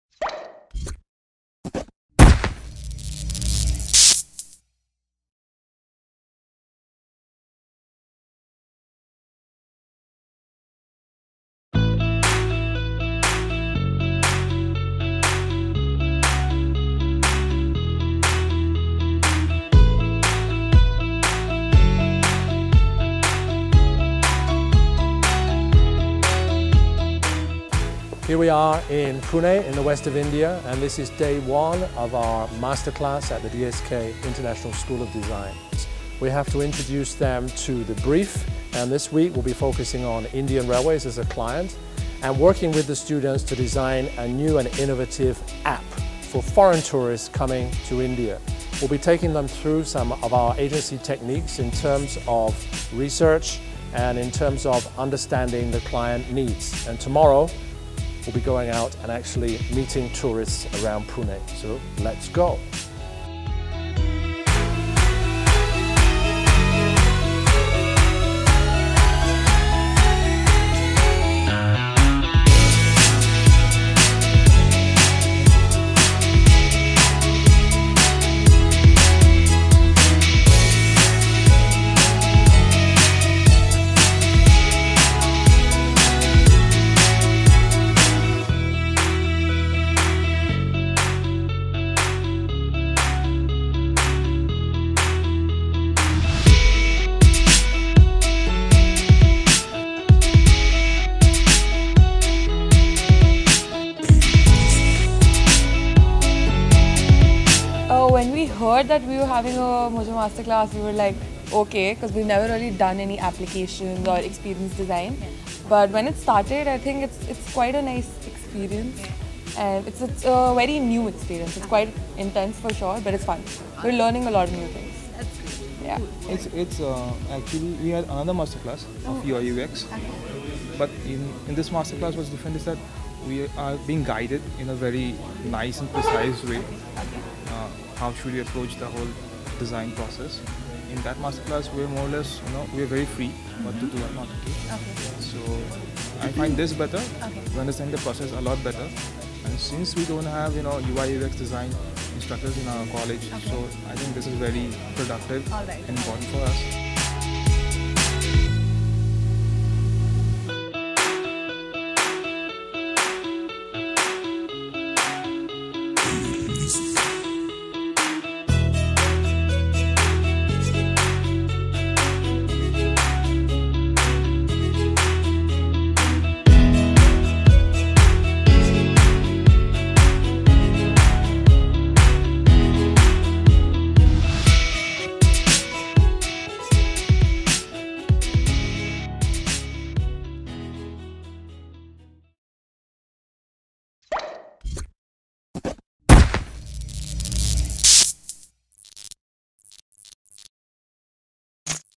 EDMOJO x DSK International Campus : APP Design Masterclass for Indian Railways
Team Ed.Mojo recently concluded a week long app experience design masterclass at the DSK International Campus in Pune. This in-depth workshop saw students develop innovative and reliable application concepts aimed at enhancing the travel experience of foreign tourists using Indian Railways.  Students presented their ideas through TVCs, staged exhibitions and prototypes.  The team also interviewed them for an article on design education in India to be published by a leading education newspaper.